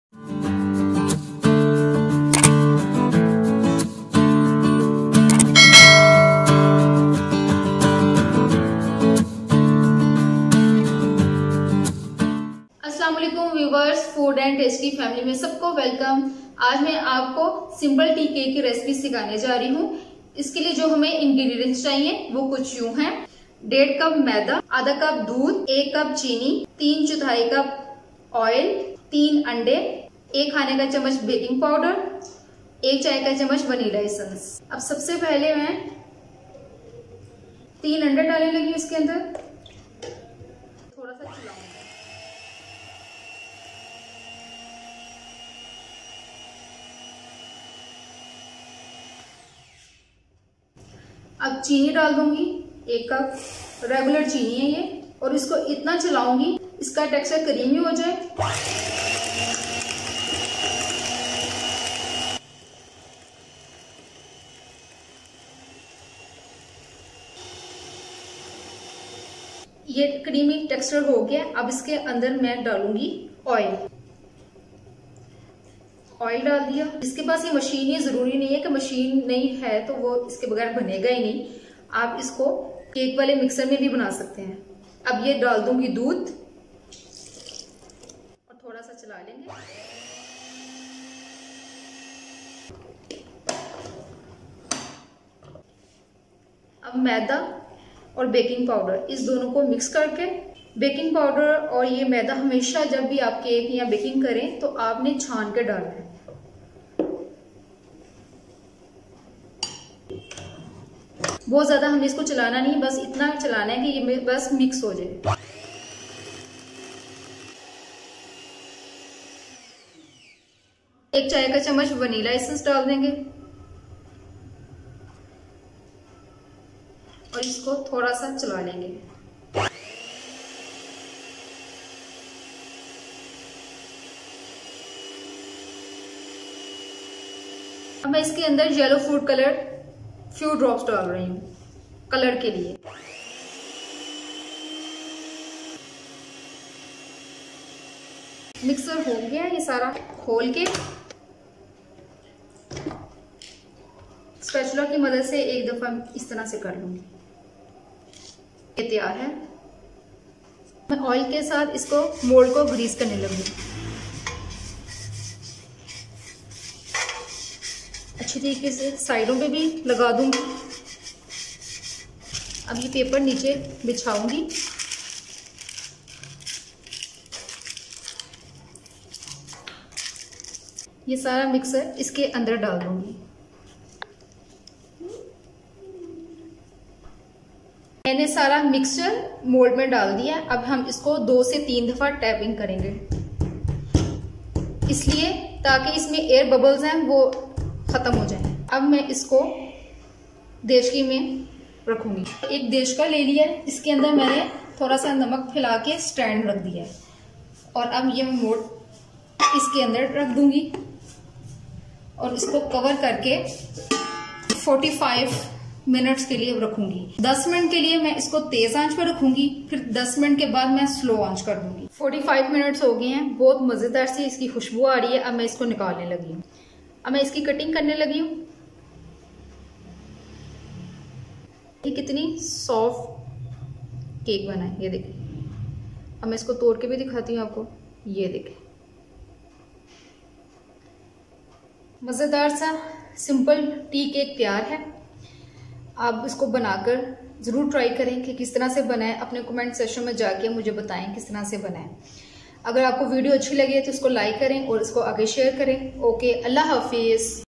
Tea Cake Chai Wala Cake Fast & Easy With & Without Oven Recipe
How to make a Soft Tea Cake Without Oven at Home for Tea Time. A Plain Cake Recipe for Tea Time by Food & Taste.